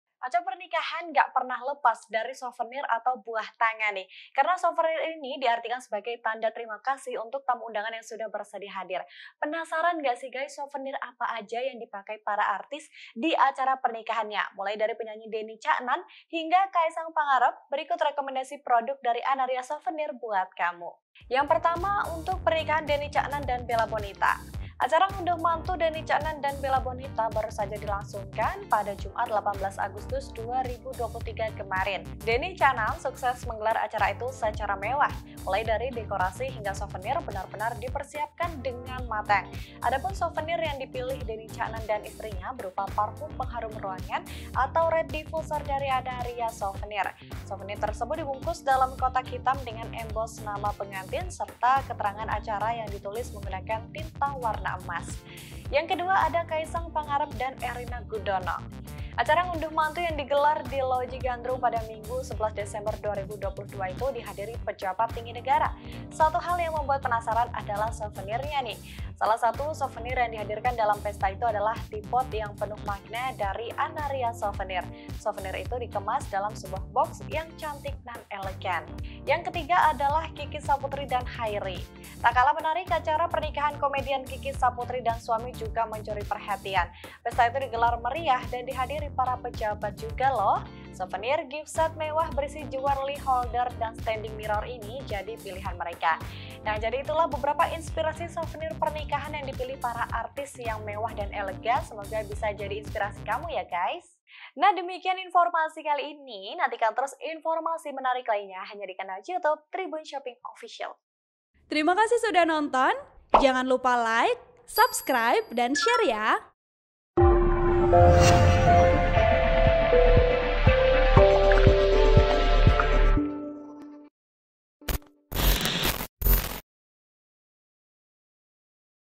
Inilah Rekomendasi Souvenir Elegan & Mewah Pilihan Artis DENNY CAKNAN hingga KAESANG PANGAREP
TRIBUN-VIDEO.COM - Acara pernikahan nggak pernah lepas dari souvenir atau buah tangan.

Karena souvenir ini diartikan sebagai tanda terima kasih untuk tamu undangan yang sudah bersedia hadir.

Penasaran nggak sih souvenir apa saja yang dipakai para artis di acara pernikahannya.

Mulai dari penyanyi Denny Caknan hingga Kaesang Pangarep, berikut rekomendasi produk dari Anaria Souvenir untukmu.

Beli rekomendasi souvenir pernikahan melalui link berikut ini:

1. Souvenir Hampers BUTTERFLY Soap Dispenser Giftbox

2.  Souvenir Mewah dan Eksklusif FLORENCE SET

3. Souvenir Wedding Kado Pernikahan CLARETTA SET